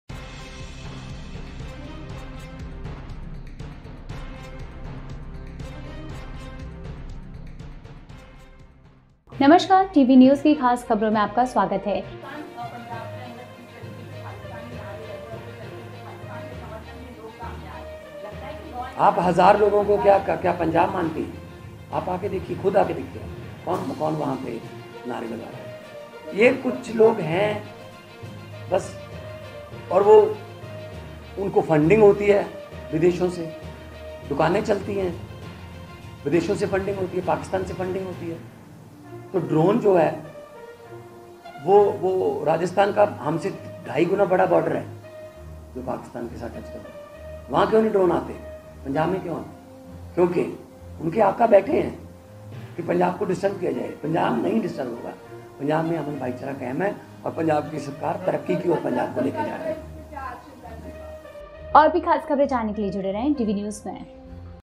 CM Bhagwant Mann: Do you consider 1000 people as Punjab? | क्या आप 1000 लोगों को पंजाब मानते हैं?
खालिस्तानी हमदर्द अमृतपाल सिंह और उनके समर्थकों को लेकर पंजाब में हालिया उथल-पुथल के मद्देनजर, सीएम भगवंत मान कहते हैं, "क्या आप 1000 लोगों को पंजाब मानते हैं? यह विदेशी, पाकिस्तान द्वारा वित्त पोषित कुछ लोग हैं ... उनके आका परेशान करना चाहते हैं पंजाब..."

In wake of the recent turmoil in Punjab regarding Khalistani sympathiser Amritpal Singh & his supporters, CM Bhagwant Mann says, "Do you consider 1000 people, Punjab? It's just a few people funded by foreign, by Pakistan...Their masters want to disturb Punjab..."